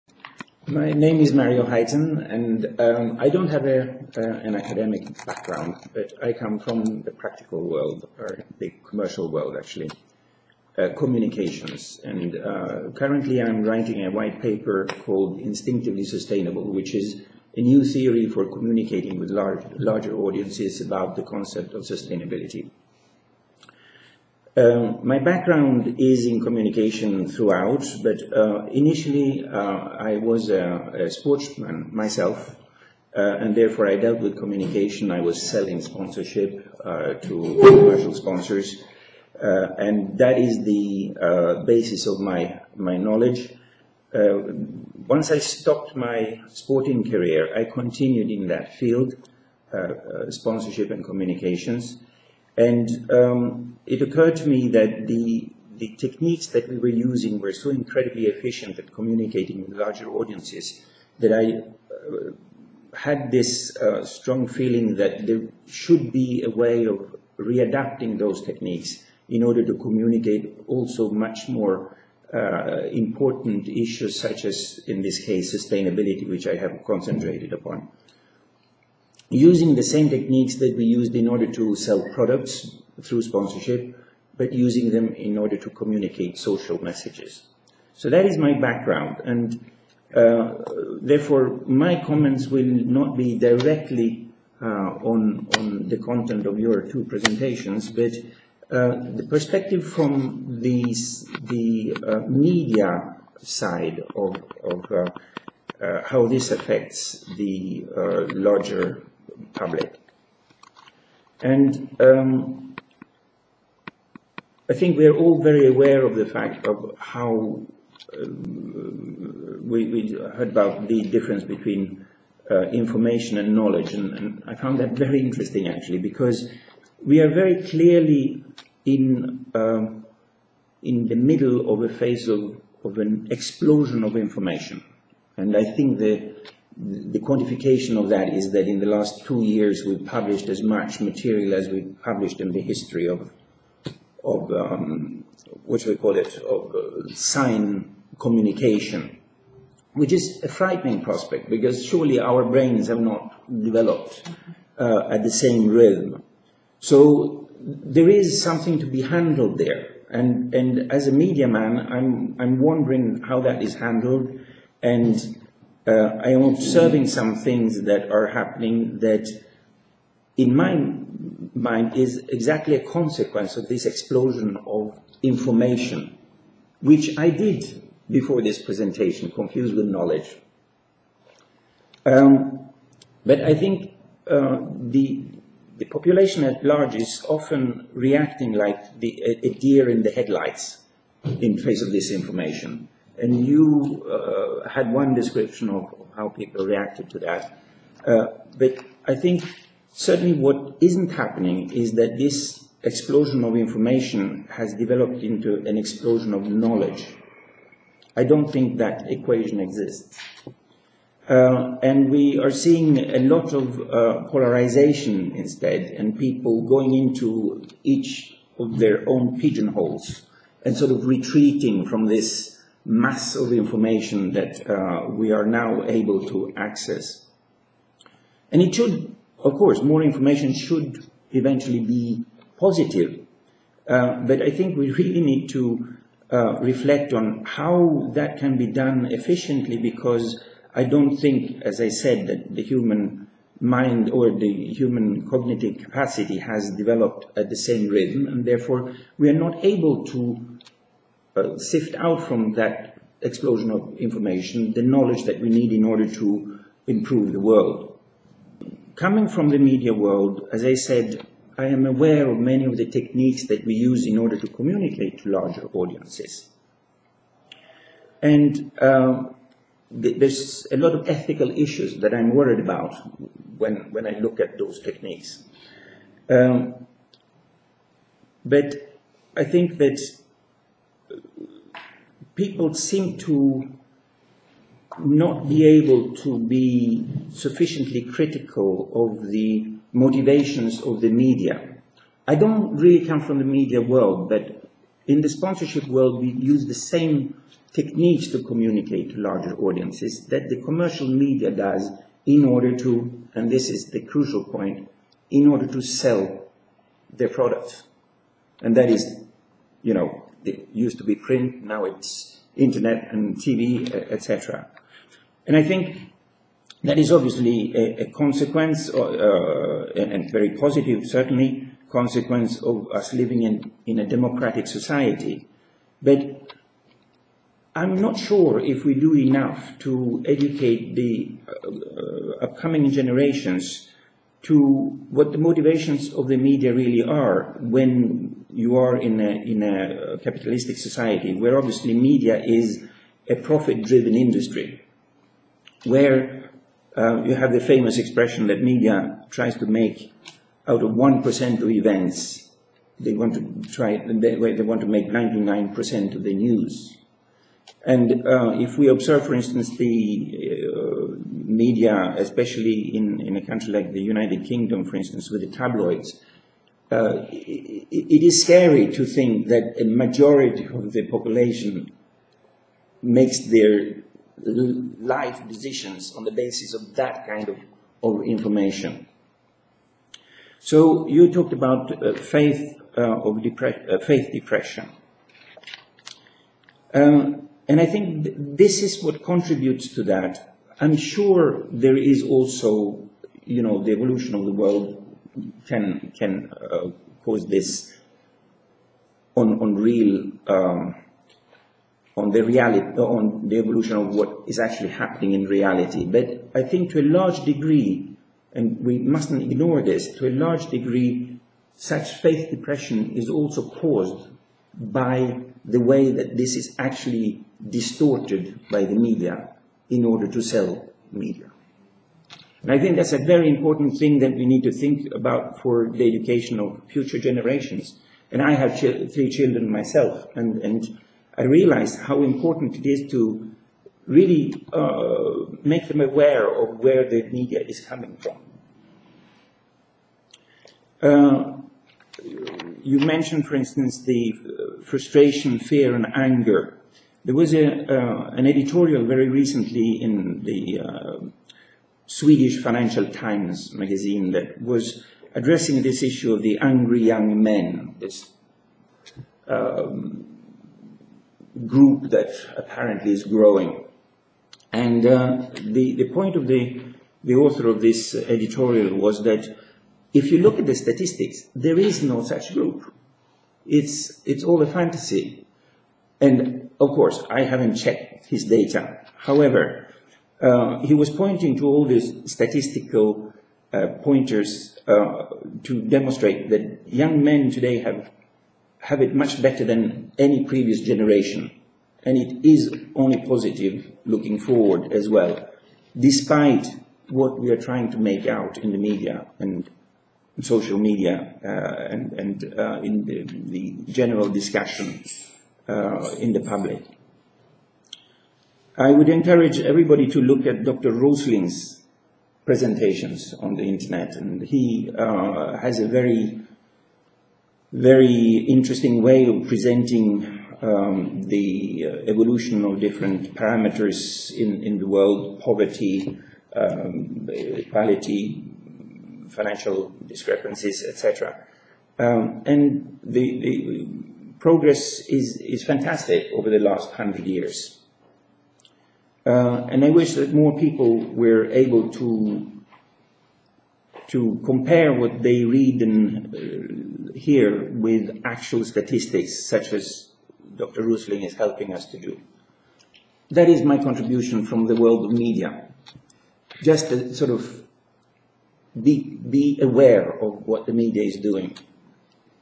Power of knowledge | Mario Hytten | Course on Mind, Thinking & Creativity
Course on 'Mind, Thinking & Creativity' explores the characteristics of mental knowledge and thought processes, types of thinking, the character of rational thought, the mental and social construction of knowledge, deep thinking, creativity and genius.